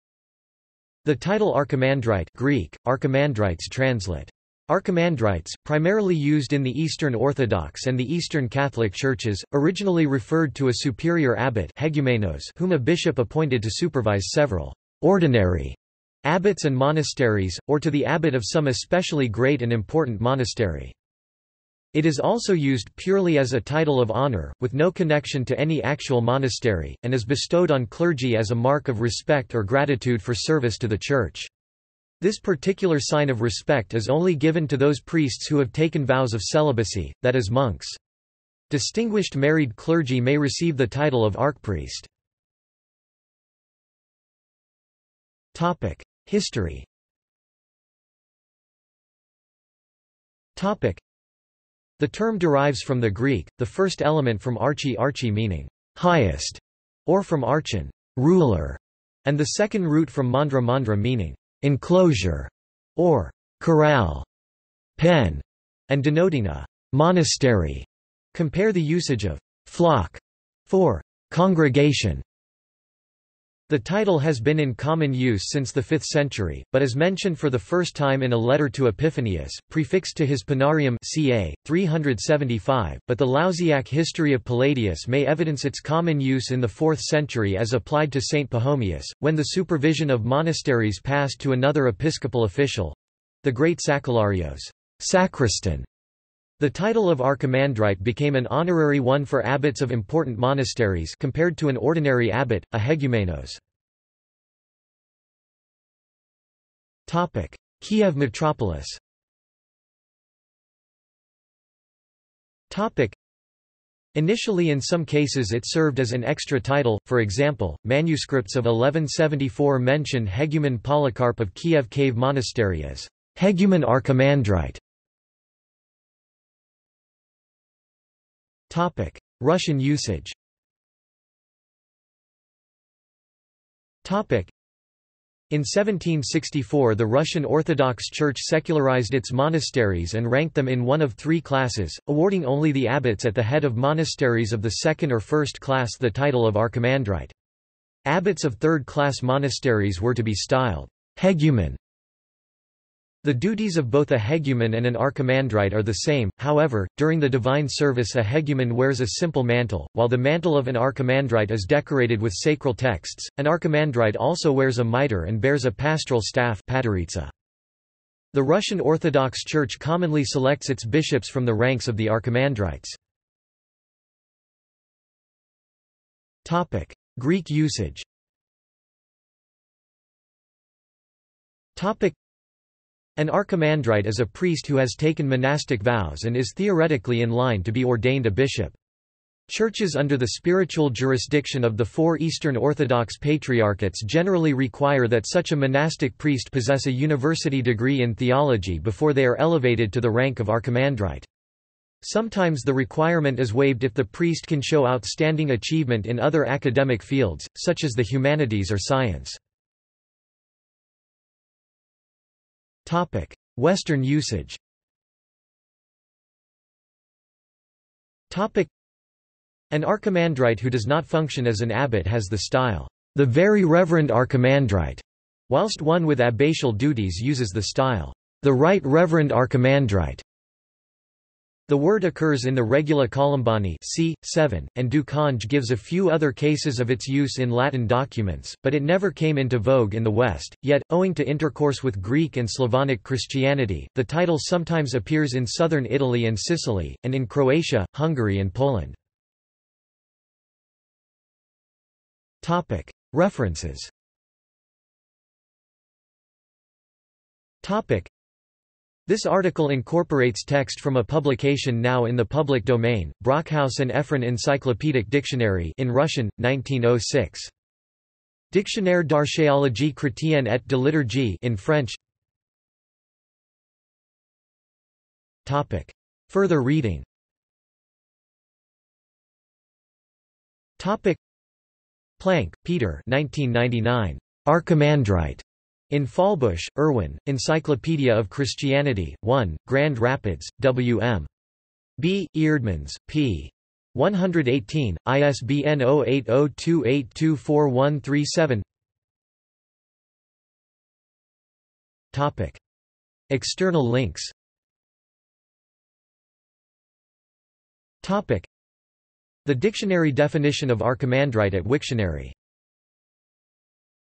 Archimandrite | Wikipedia audio article
This is an audio version of the Wikipedia Article:
Archimandrite

SUMMARY
The title archimandrite (Greek: ἀρχιμανδρίτης, translit. archimandritēs), primarily used in the Eastern Orthodox and the Eastern Catholic churches, originally referred to a superior abbot (hegumenos) whom a bishop appointed to supervise several 'ordinary' abbots and monasteries, or to the abbot of some especially great and important monastery.
It is also used purely as a title of honour, with no connection to any actual monastery, and is bestowed on clergy as a mark of respect or gratitude for service to the Church. This particular sign of respect is only given to those priests who have taken vows of celibacy, that is monks. Distinguished married clergy may receive the title of archpriest.